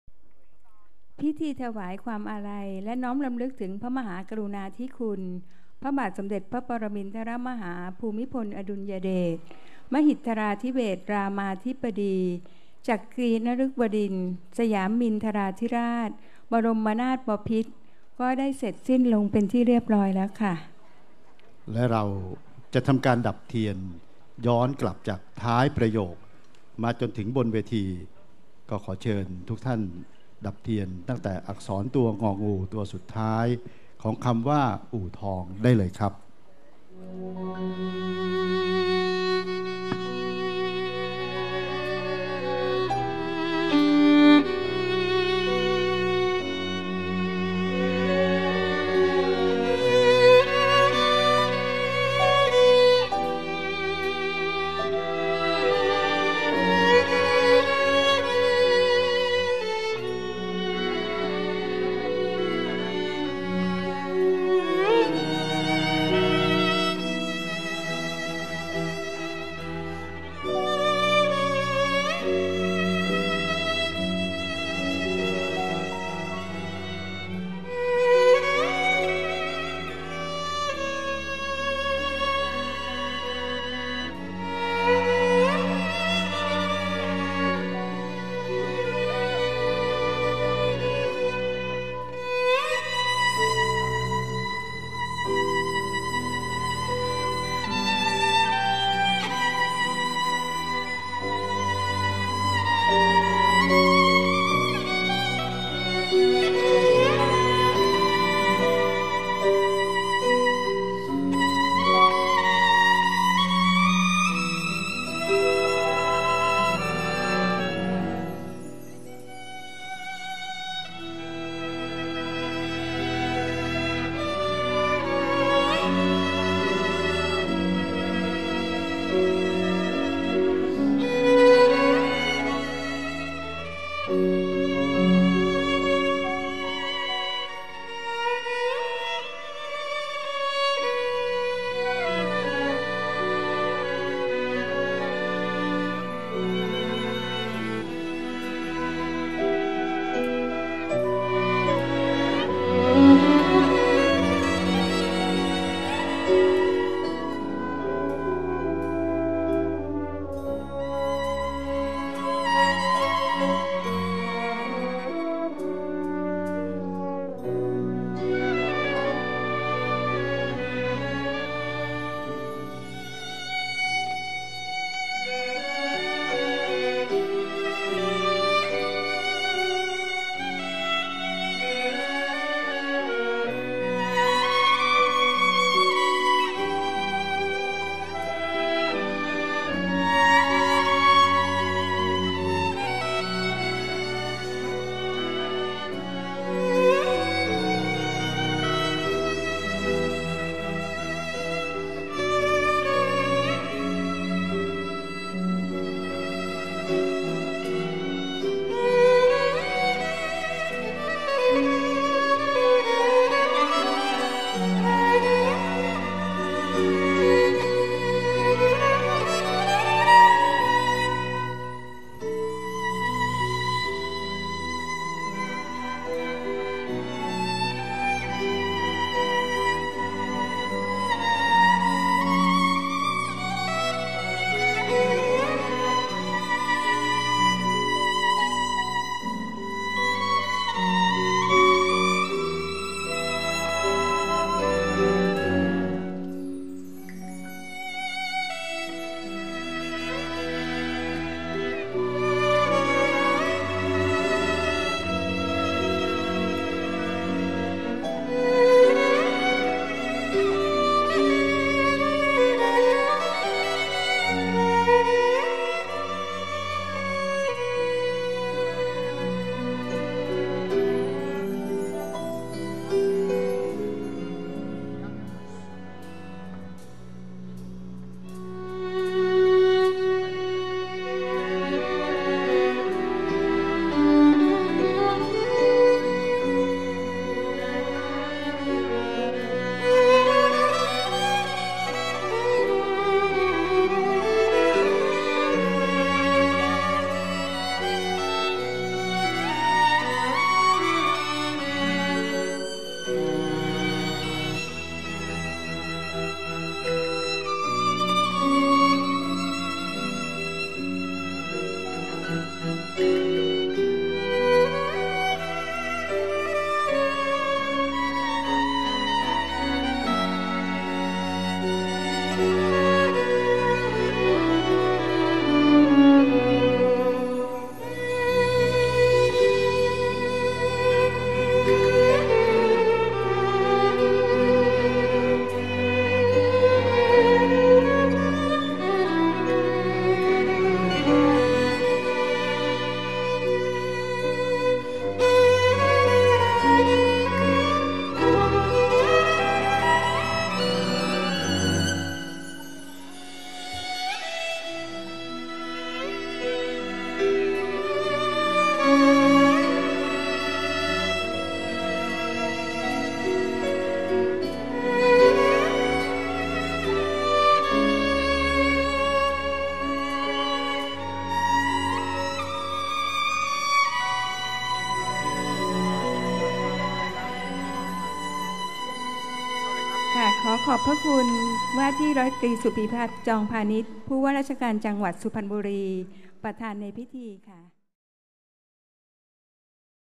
การดับแสงเทียนของชาวอู่ทอง อาทิตย์ดับแสง
การดับเทียน จากอักษรตัวสุดท้าย เป็นไอเดียของท่าน นายอำเภอชนพหล ส่งเสริม นายอำเภอคนเก่งของเรา และ โดยคณะทำงานของคุณพชรพรรณ มาตรศรี (คุณแก้ว) นำทีมทำงานเพือถวาย ในหลวงของพวกเรา จัดเตรียมงานเป็นระยะเวลากว่าเดือนเพื่อให้ออกมาอย่างสมพระเกียรติที่สุด